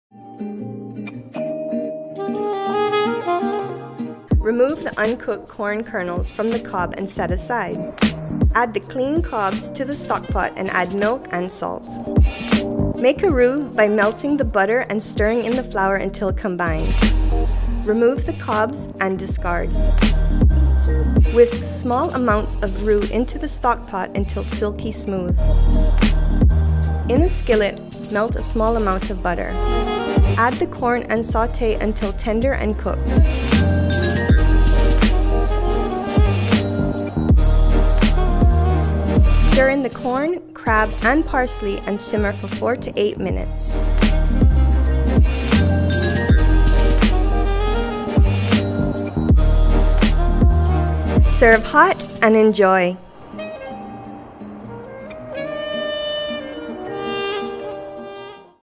Crab & Corn Chowder
You guys, this crab & corn chowder is SO yummy, and perfect to get you in the mood for fall! I know, I know, we don't really have a fall, but it's going to start cooling off soon and before you know it, it's Thanksgiving and Christmas [yay]!

Ingredients
3 ears corn on the cob
4 cups milk
2 cups water
1 lb lump crab meat
¼ cup parsley
3 tbsp butter
3 tbsp flour
salt & pepper

Directions
Remove the uncooked corn kernels from the cob and set aside. Add the cleaned cobs to a stock pot and add milk, water and salt. Bring to a boil. Reduce to a simmer and cook for 15-25 minutes. Remove the cobs and discard.
• Make a roux by melting the butter and stirring in flour until combined. Cook for 2-4 minutes until the mixture is cooked through but not dark in color.
• Whisk small amounts of roux into the hot stock until all incorporated and the stock becomes silky smooth. Season with salt and pepper and let simmer.
• In a skillet on medium high heat, melt a small amount of butter. Once the butter has melted, add the corn and sauté until tender and cooked. Season with salt and pepper.
• Drain the crab meat and chop the parsley. Stir in the corn, crab and parsley and simmer for 4-8 minutes. Serve hot.